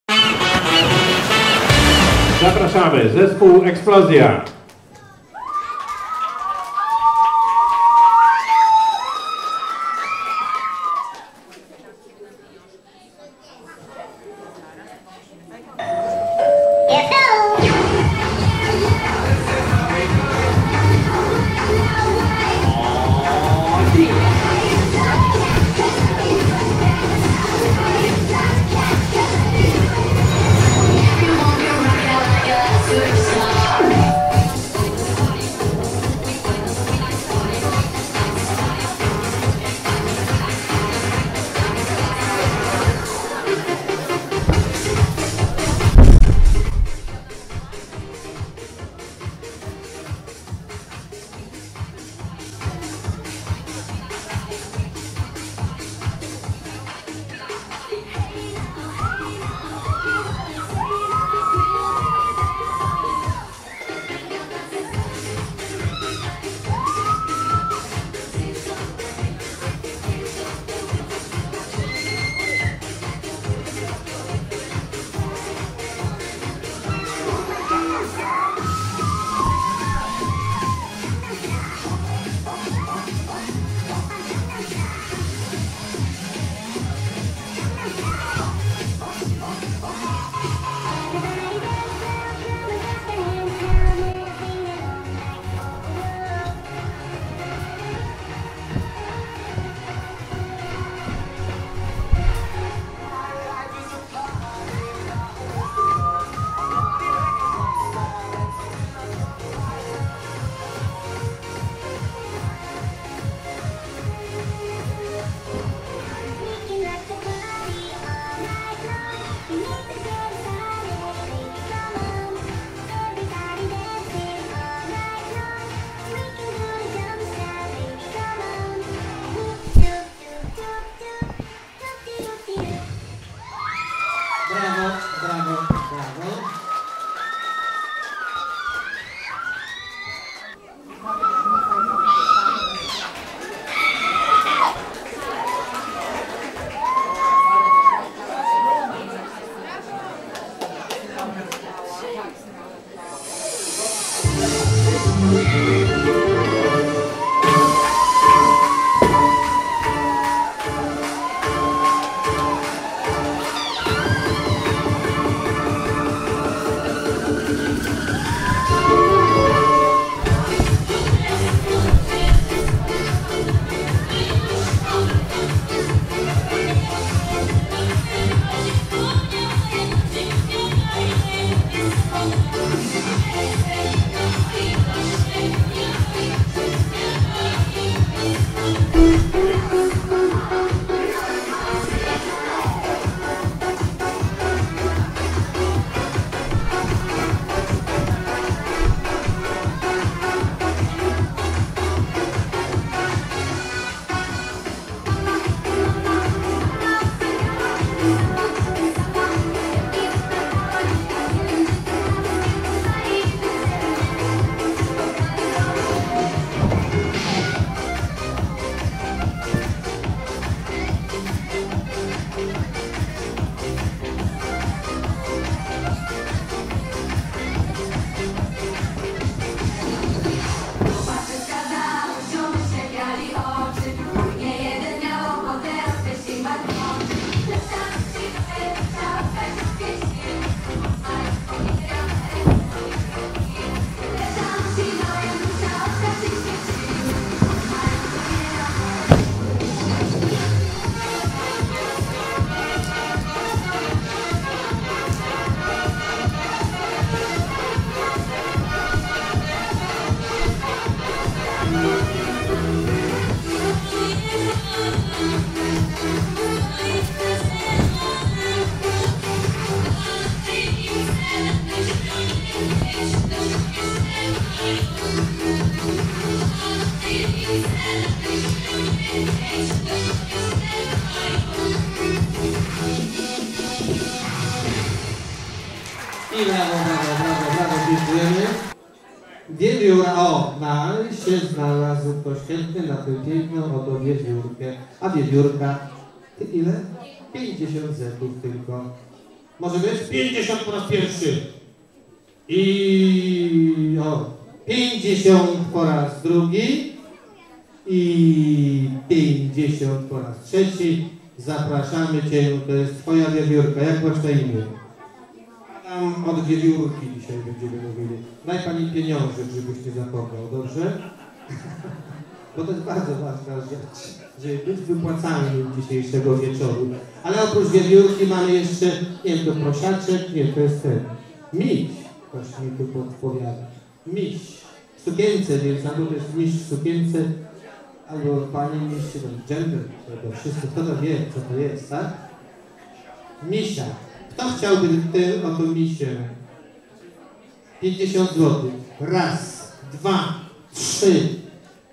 MDK w Kałuszynie zaprasza na występ - 28 Finał Wielkiej Orkiestry Świątecznej Pomocy - EKSPLOZJA
W dniu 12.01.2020 po raz kolejny graliśmy razem z Wielką Orkiestrą Świątecznej Pomocy. To dzięki wam udało się zebrać 53000 zł. Dziękujemy wszystkim zaangażowanym w organizację koncertu, wszystkim kwestującym. Wielkie słowa uznanie dla występujących, dla tych małych i dla tych dużych. Bardzo dziękujemy wszystkim dobroczyńcom, bez was to by się nie udało. Do zobaczenia za rok.

Zdjęcia:
Artur PILO & Marcin Ciechański
Montaż:
Artur PILO
Wszelkie prawa zastrzeżone
Dom Kultury w Kałuszynie 2020